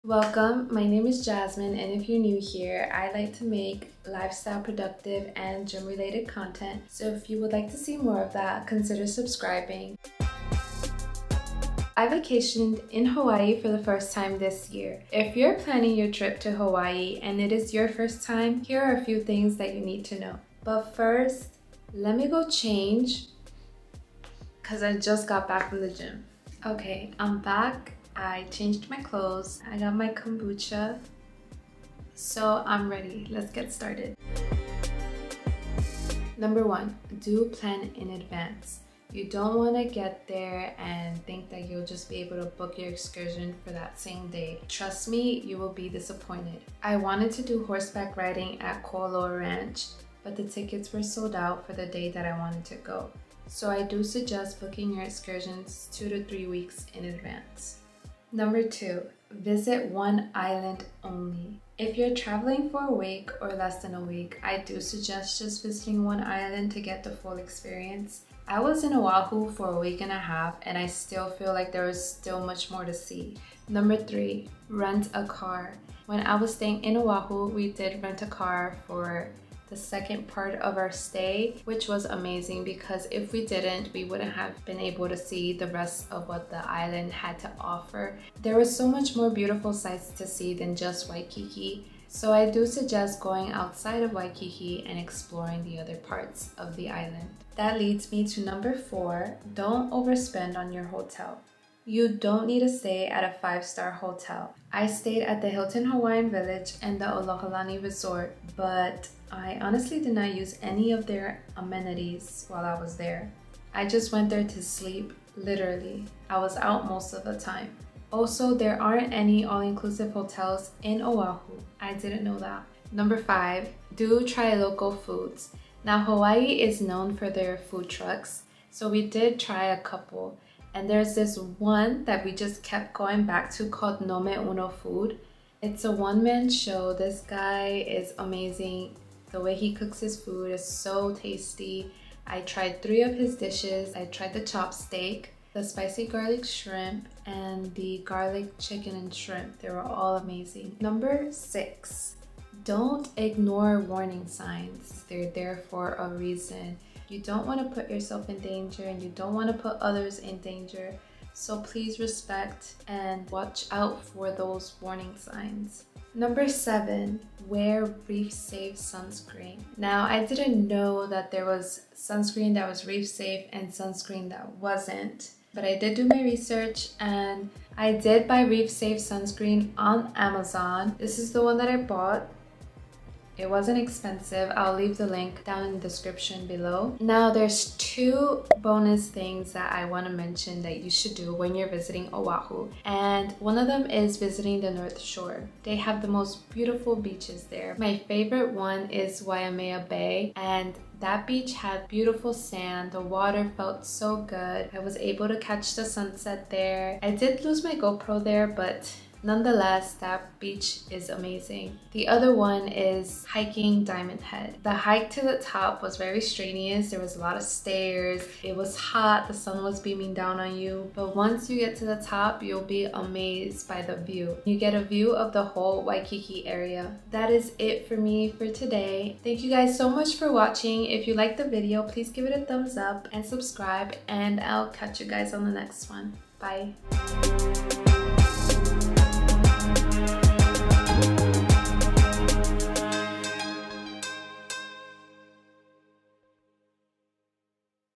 First time visiting Hawaii? 7 Things You Need to Know (Locations you need to visit)
Hi Everyone! Welcome Back. I visited Hawaii for the first time this year and there's some things I wish I knew before going. So I am here to let you know what you need to know before booking that flight to Hawaii. I hope this helps.

My heart goes out to everyone who have suffered from the Maui wildfires. If you would like to support please see the links below to find where you can donate to help Maui.

Hawaii Community Foundation Maui Strong Fund

Maui Food Bank

Maui United Way (direct relief to families and non-profits)

📍Featured:
Nome Ono Food
Ceviche House Hawaii Peruvian Cuisine
Kualoa Ranch
Waimea Bay
Keawaula Beach
Diamond Head
Waikiki
Halona Blowhole lookout
Hilton Hawaiian Village
Alohilani Resort
Byodo-In Temple

Reef safe sunscreen: Blue Lizard SENSITIVE Mineral...